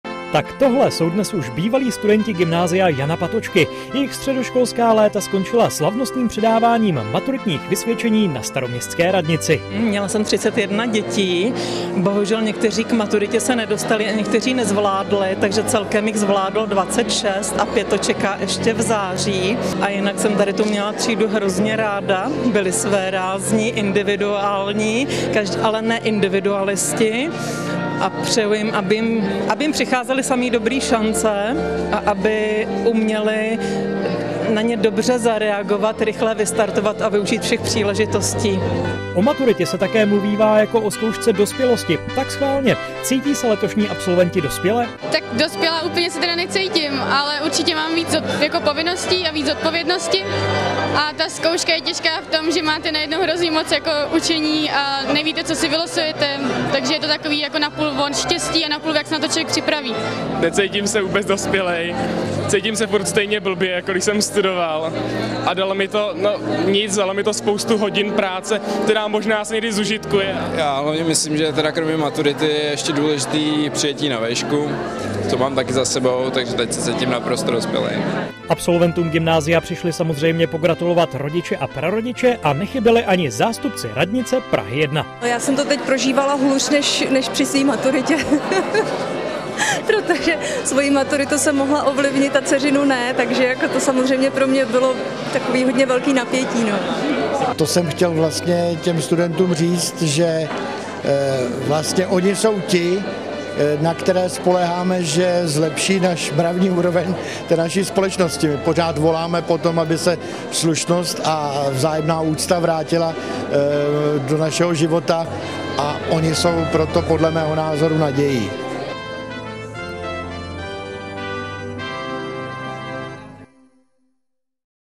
Gymnázium Prof. Jana Patočky se s absolventy loučilo na Staroměstské radnici
Studenti Gymnázia Prof. Jana Patočky přebírali svá maturitní vysvědčení na Staroměstské radnici. Zkoušku dospělosti sice berou jako důležitou metu, ale zároveň si uvědomují, že to hlavní mají stále před sebou. Většina z nich se teď soustřeďuje hlavně na přijímací zkoušky na vysokou školu.